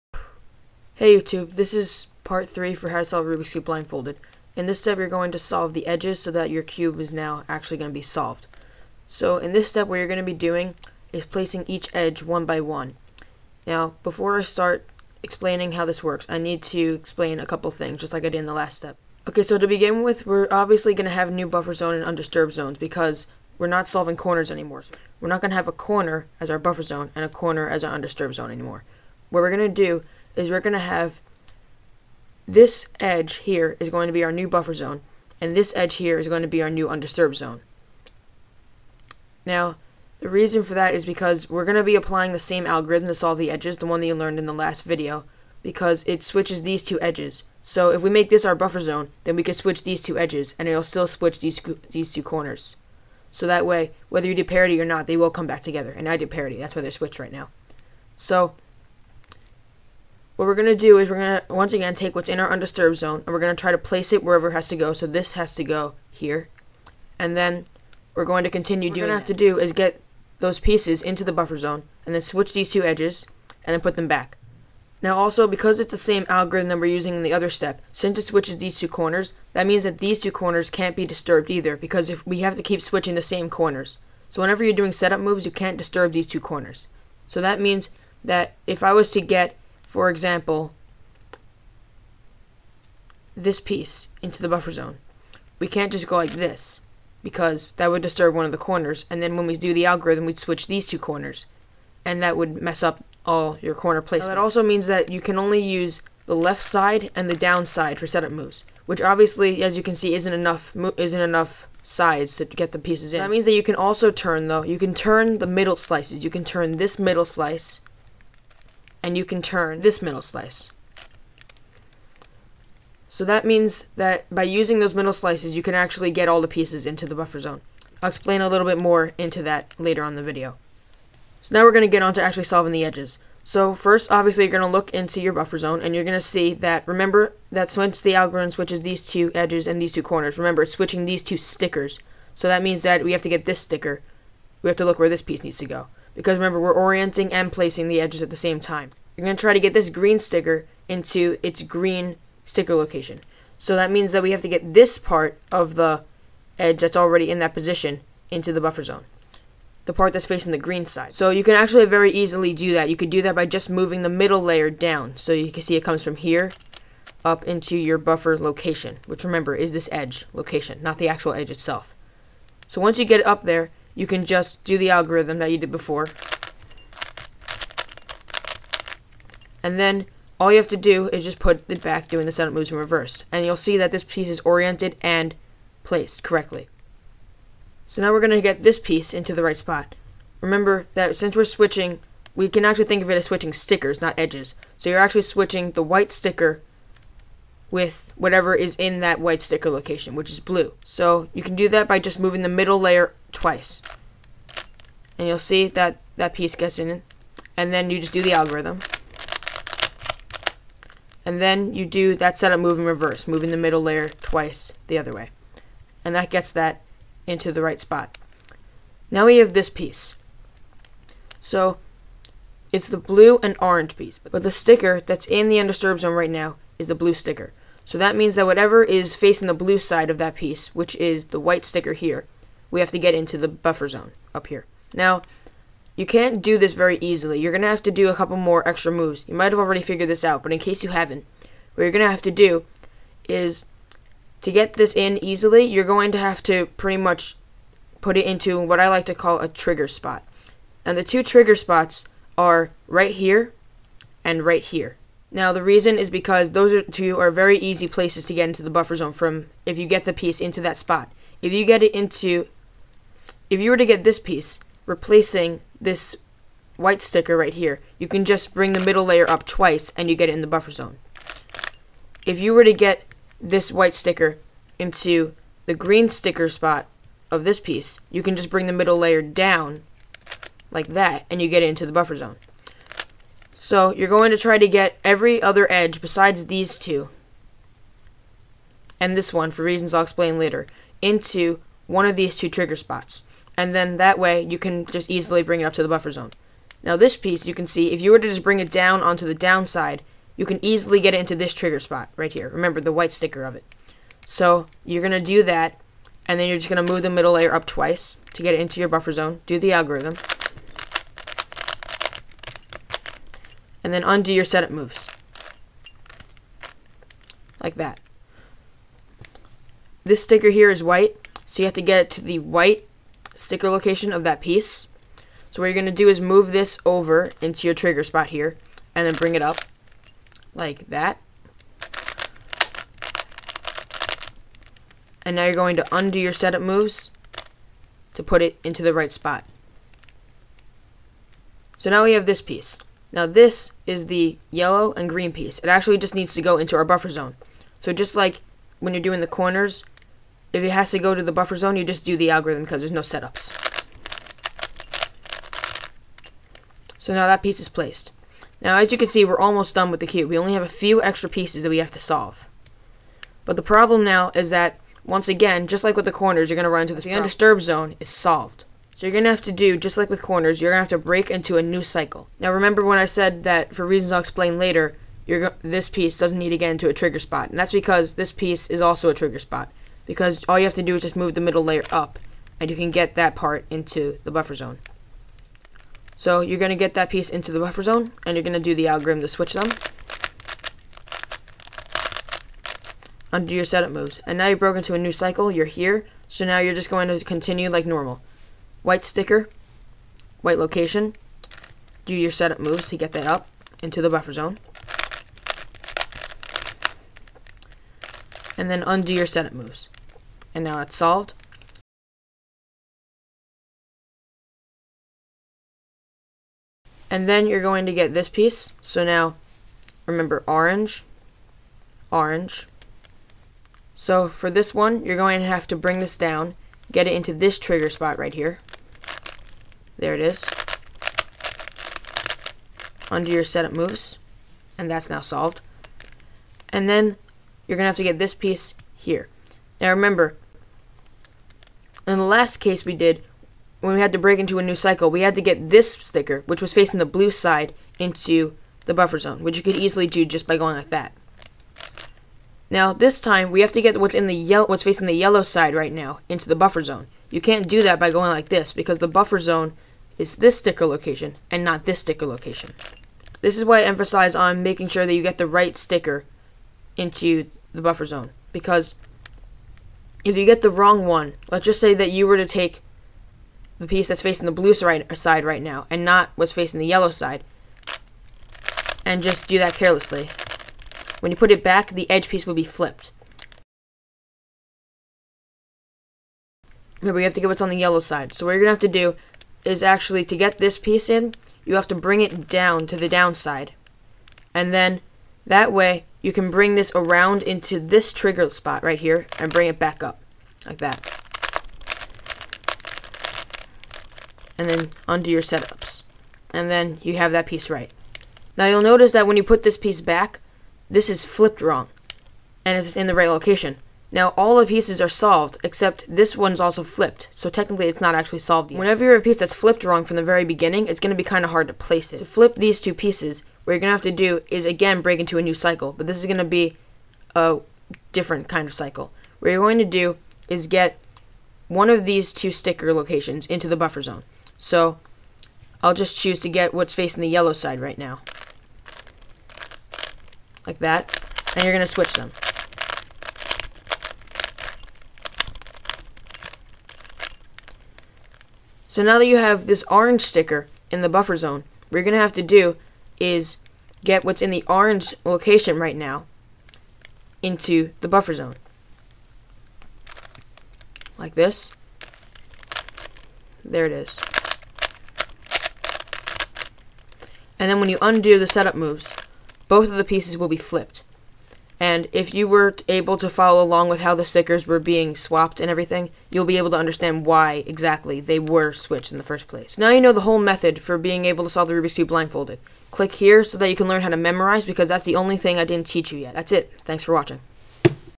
How to Solve a Rubik's Cube Blindfolded (Part 3)
This is the revised version of this step, where you learn how to solve the edges. I recommend watching this a few times because this can be confusing.

Algorithm: L U' R' U L' U2 R U' R' U2 R